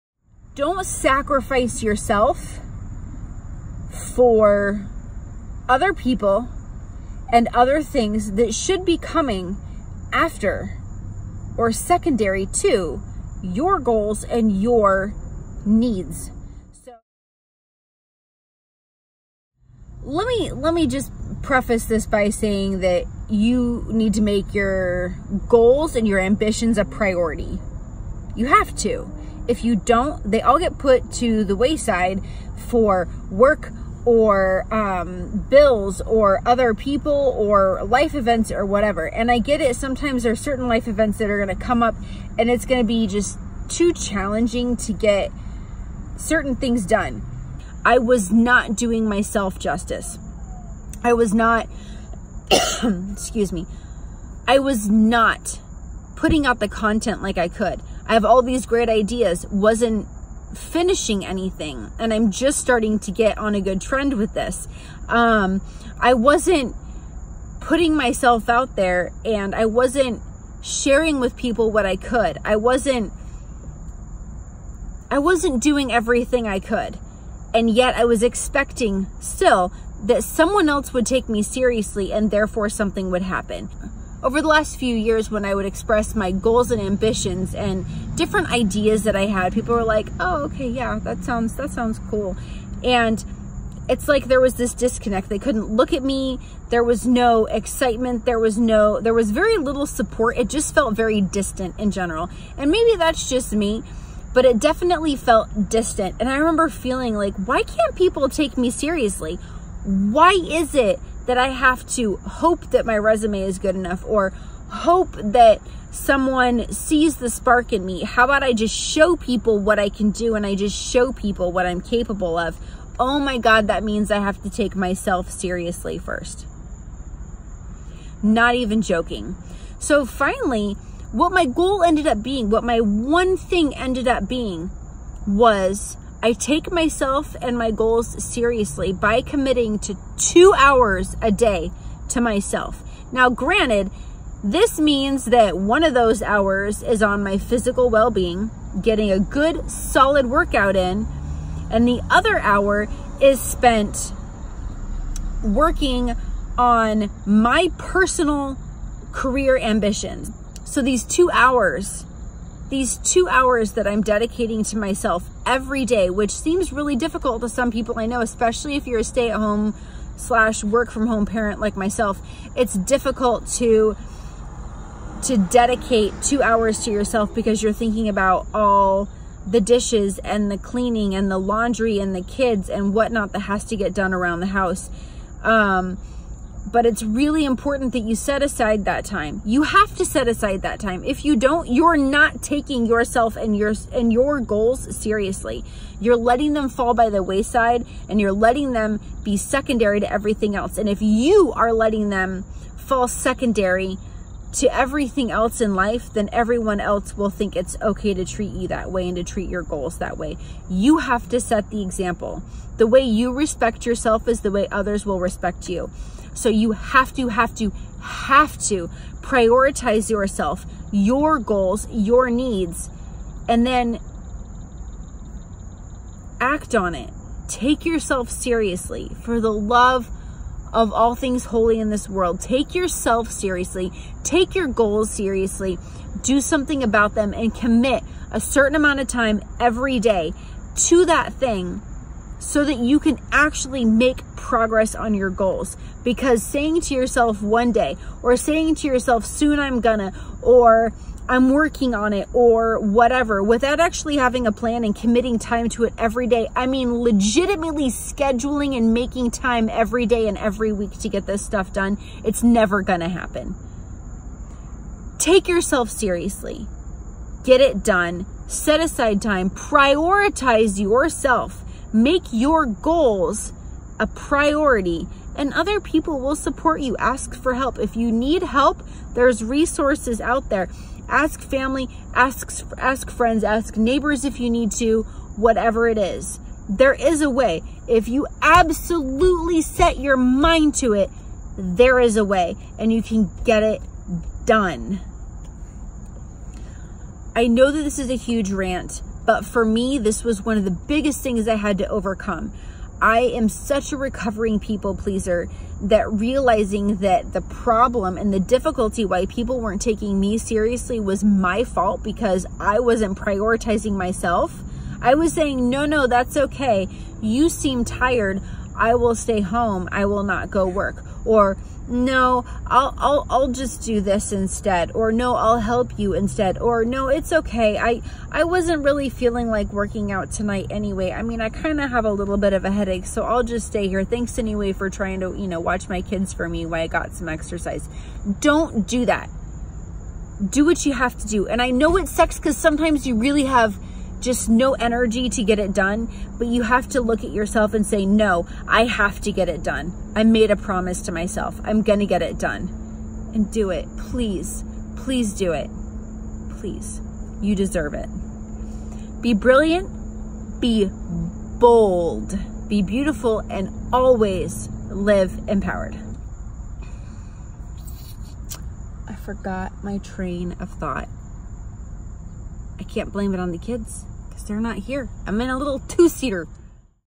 DO YOU TAKE YOURSELF SERIOUSLY??!?
Why is it so difficult for some of us to put ourselves, our goals, our wishes and needs after others? We MUST put ourselves first. We are all we have, all our families have, all our friends have, all our kids have. Our body, our goals and dreams are for us and us alone to fight for otherwise they will never see the light of day. Get moving on your goals! Get fit. Get educated, via on your own or otherwise. Make time for you and don't break that promise! You deserve it!